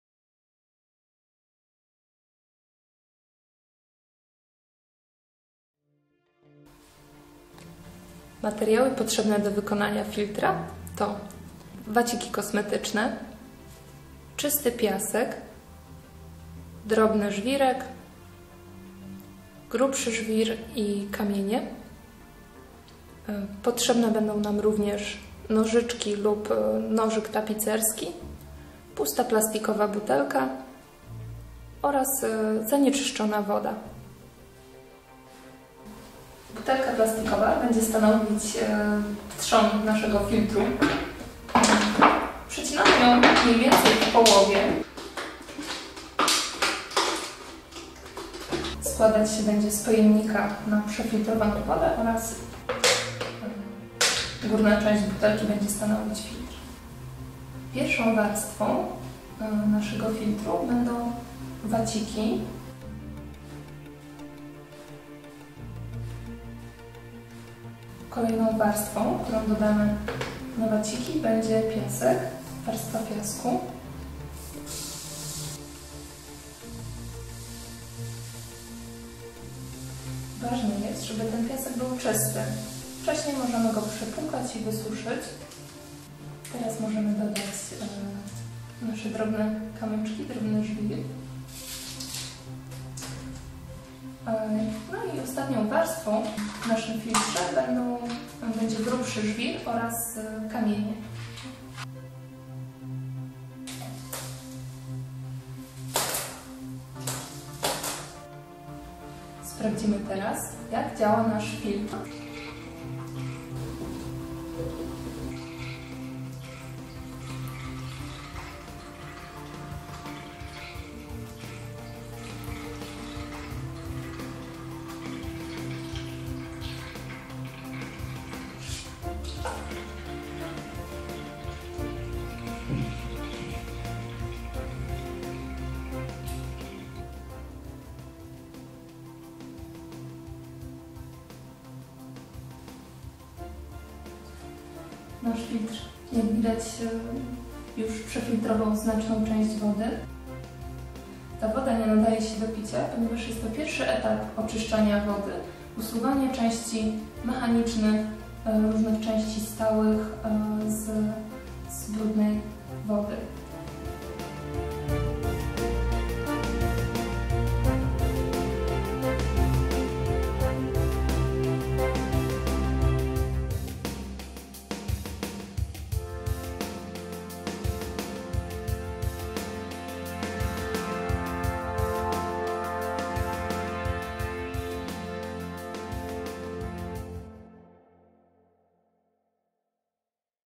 Filtr wodny
Dzięki takiemu prostemu filtrowi można pozbyć się wielu zanieczyszczeń. Jednak rozpuszczone w wodzie farby, proszki do prania, kosmetyki, środki do sprzątania (szczególnie te reklamowane jako supermocne i zabijające wszelkie bakterie) są znacznie trudniejsze do oddzielenia od wody. Warto używać ich jak najmniej -- nie marnować szamponów, mydeł, płynów, zastępować je jak najczęściej środkami ekologicznymi albo starymi domowymi sposobami jak gorąca woda, ocet, sok z cytryny czy soda.